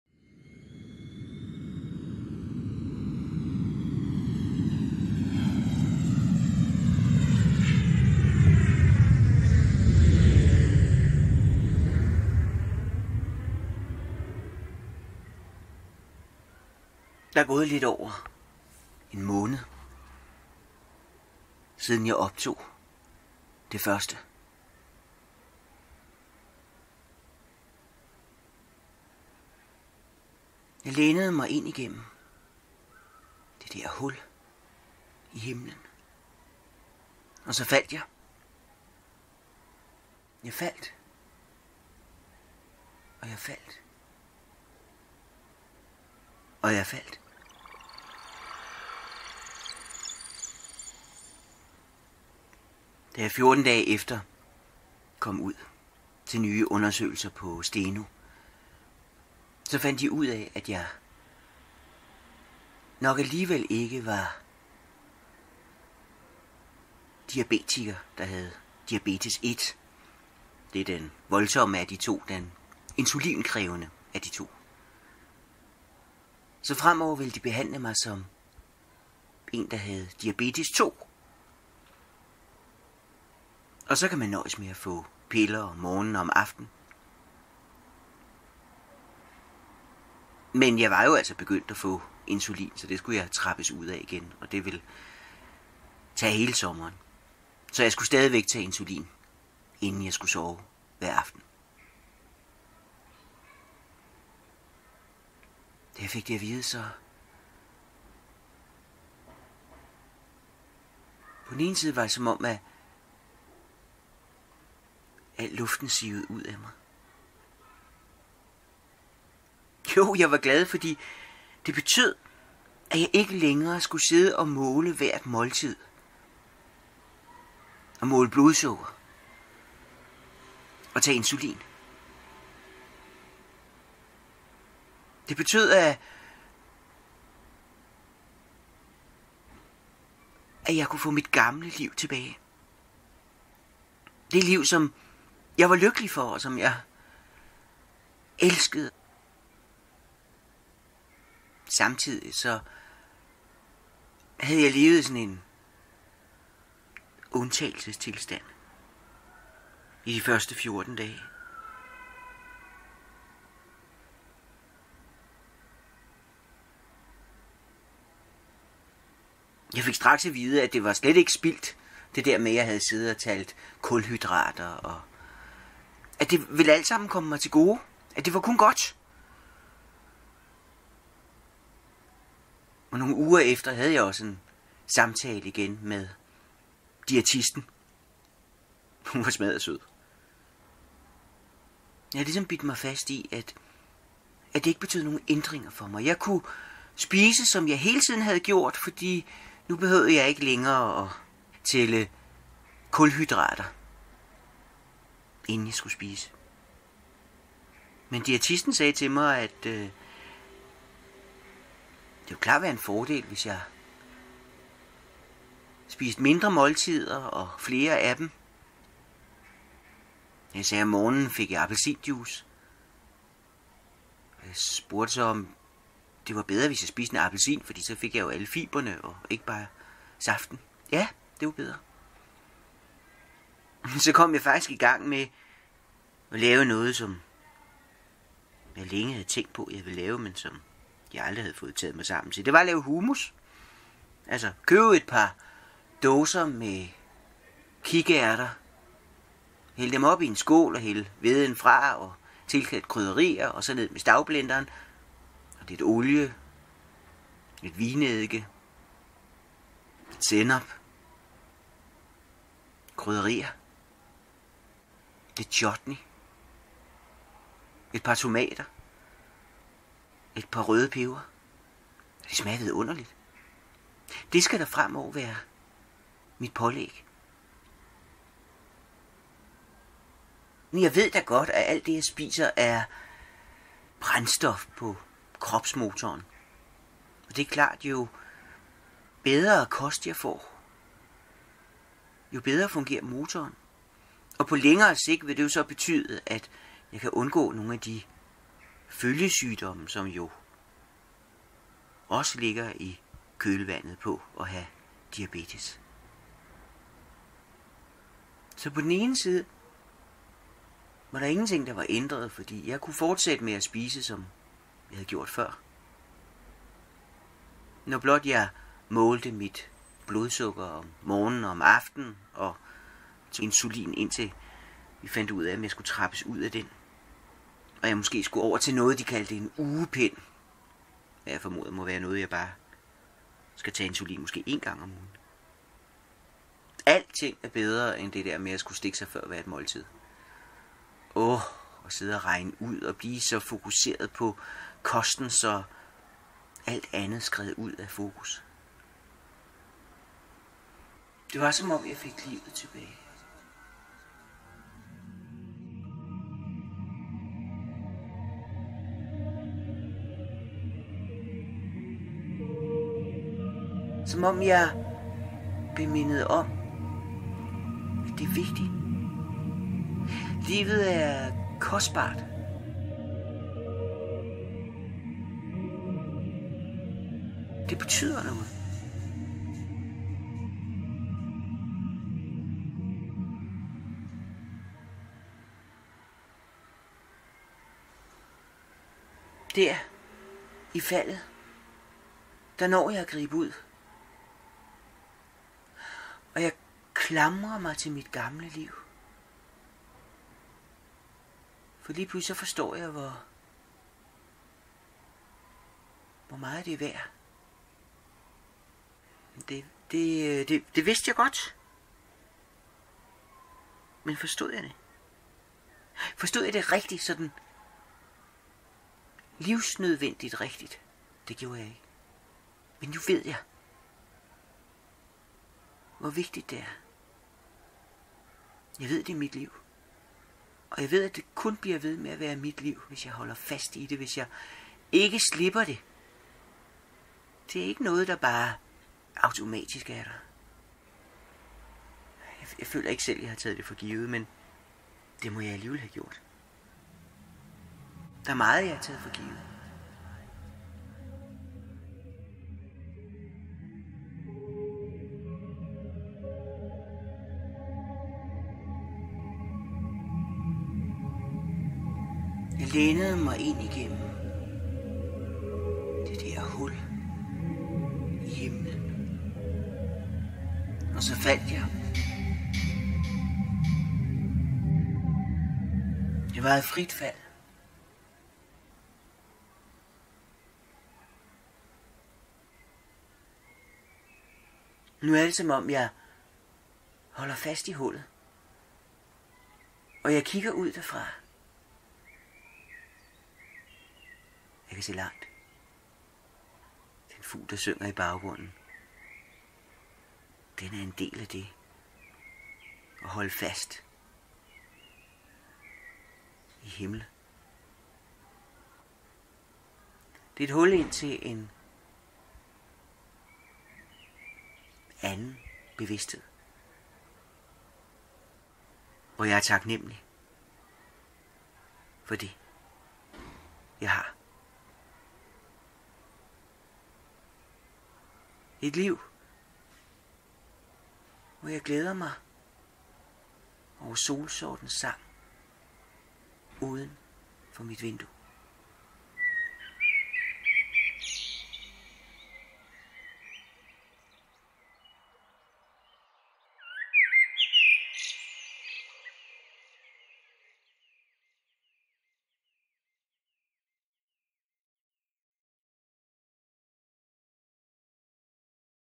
20:Thulla & Svennevg: I FRIT FALD - 2. del
ÆG & HÅB: Thulla & Svennevig: I FRIT FALD - 2. del. Hvilke tanker melder sig, når man står foran en større livsændring. Hvad sker der, når bommen går ned, og livets uendelighed forsvinder, og man er tvunget til at tage sit liv op til overvejselse. Hvor falder man hen, når man falder ned igennem et hul i himlen? Tekst: Michael Svennevig. Lyddesign: Thulla. Læs mere: Hør 1. del Læs mere om festivalen ÆG & HÅB - på Facebook